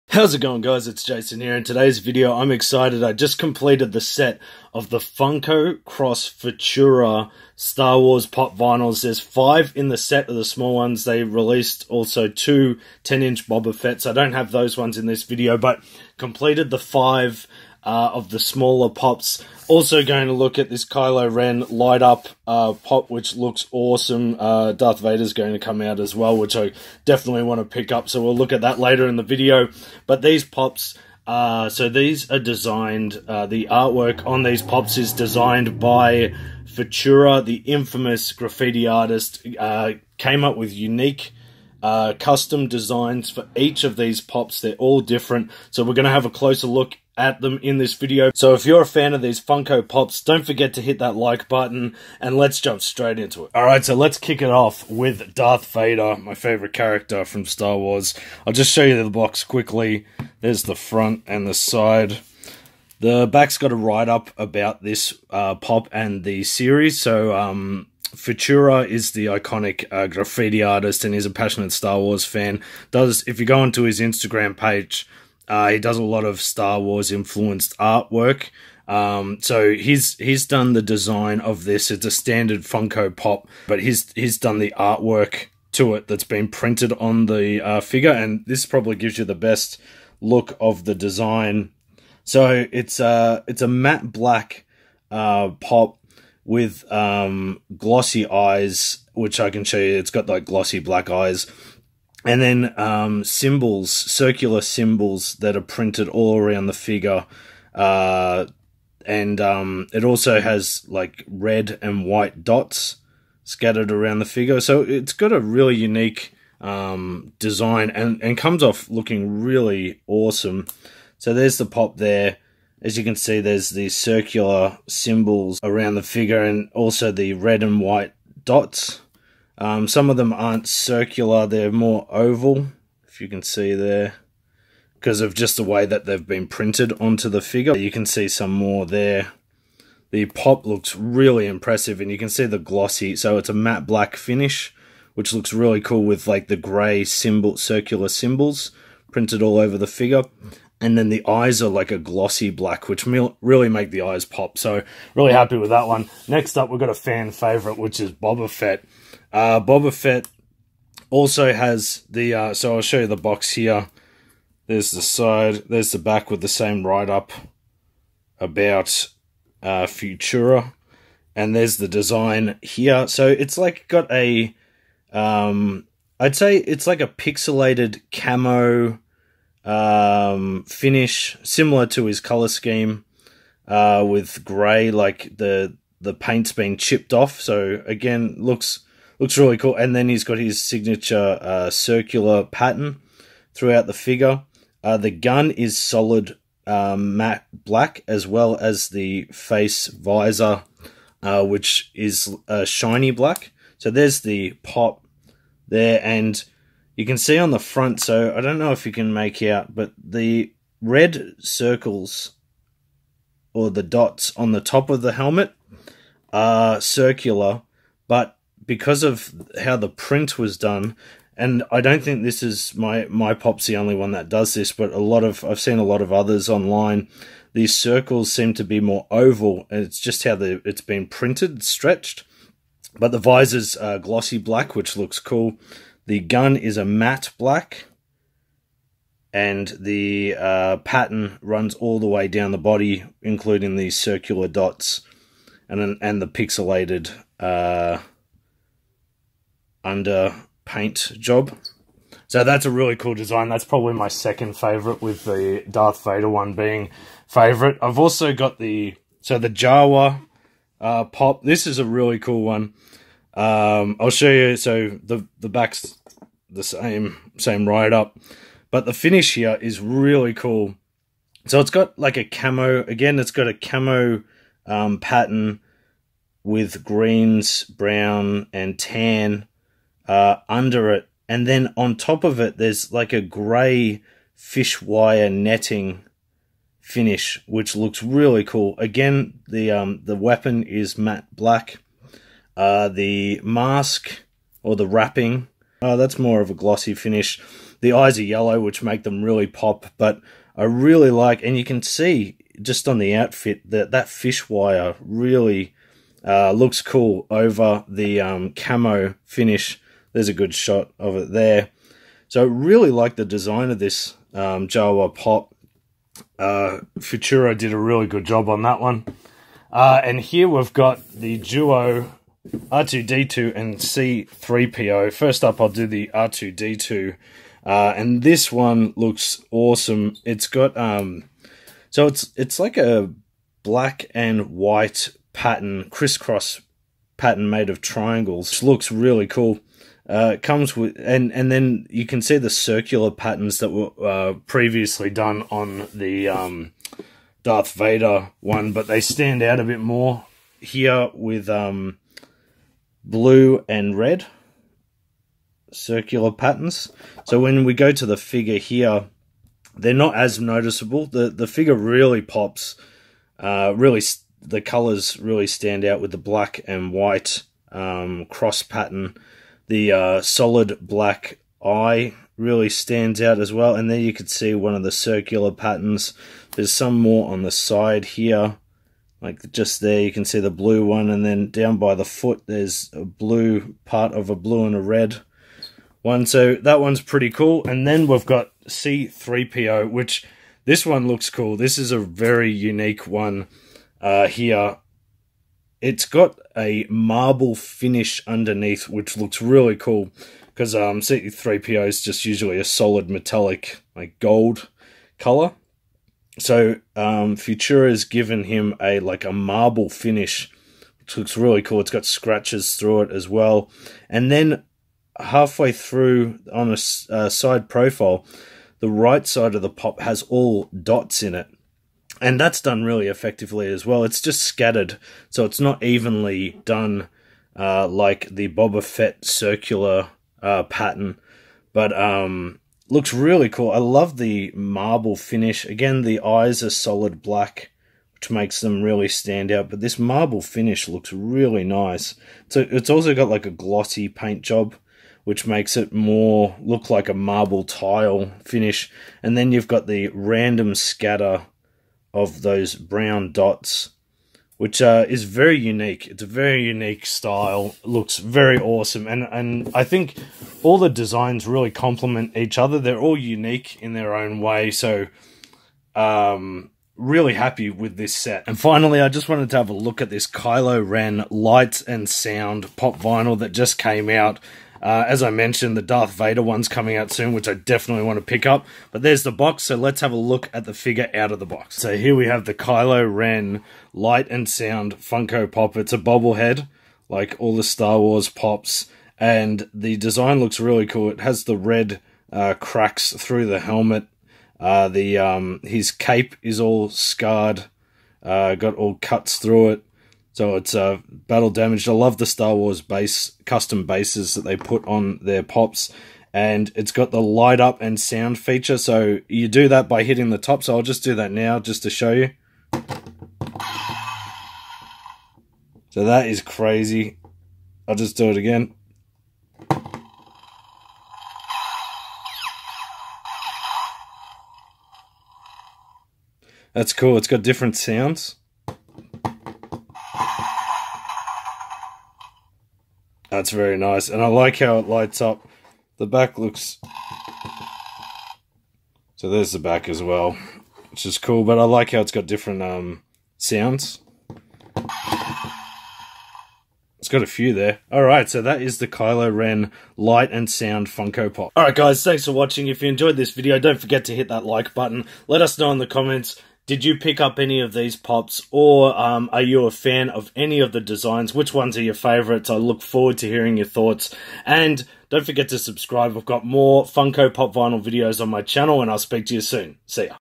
Star Wars Funko x Futura | Pop Vinyl Set
Finally completed my Star Wars Funko x Futura Pop Vinyl Set

Chase Collection | 15 Funko Pop! Vinyls -